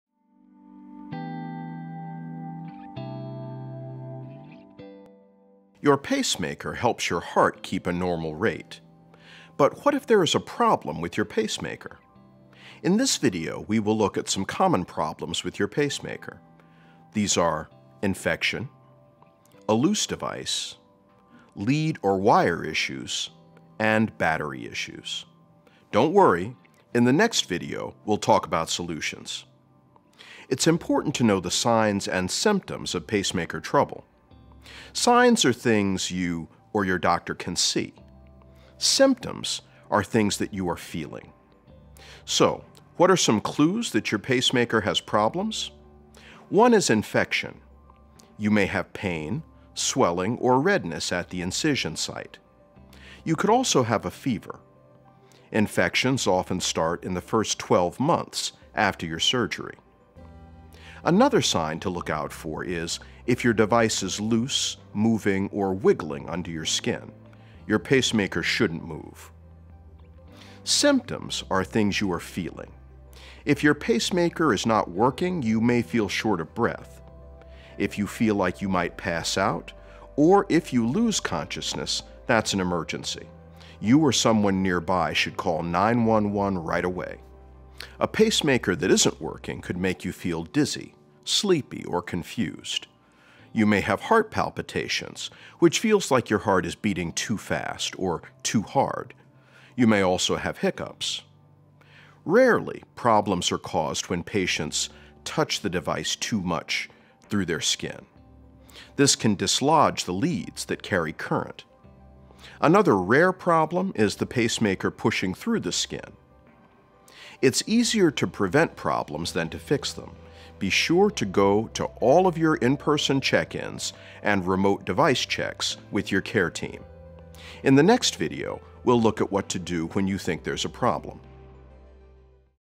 How to Know if There’s a Problem With Your Pacemaker by Mytonomy
Mytonomy’s Cloud for Cardiology has added a pacemaker placement video to its collection. Your patients will hear about the signs and symptoms of pacemaker complications. They will learn how even vague symptoms like sleepiness and confusion can indicate larger issues such as infection, a loose device, and battery issues. Education saves lives.